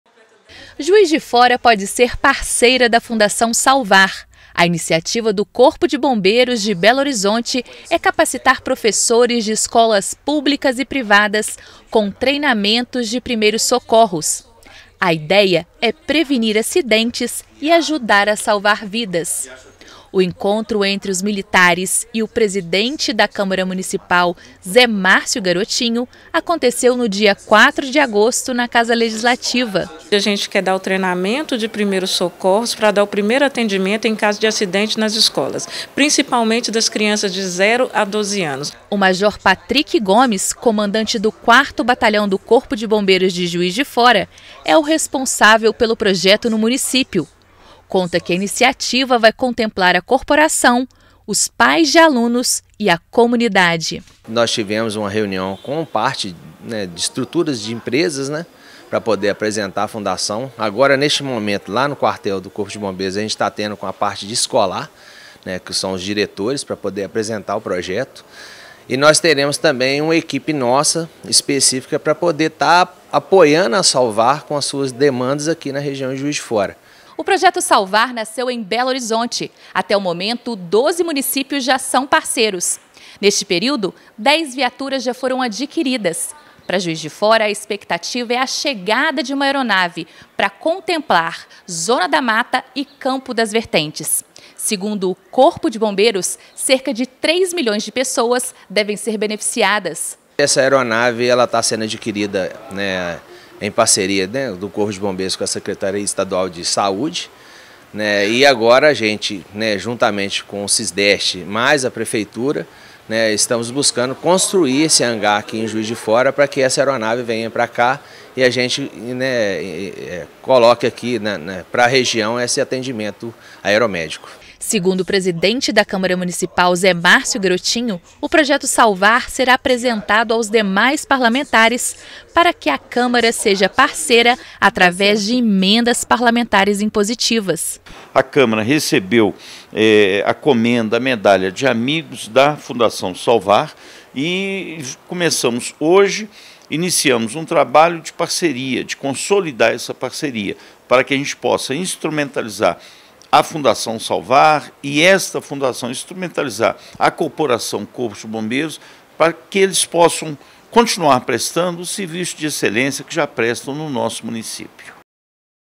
JUIZ DE FORA PODE SER PARCEIRA DA FUNDAÇÃO SALVAR
INICIATIVA DO CORPO DE BOMBEIROS DE BELO HORIZONTE  É CAPACITAR PROFESSORES DE ESCOLAS PÚBLICAS E PRIVADAS COM  TREINAMENTOS DE  PRIMEIROS SOCORROS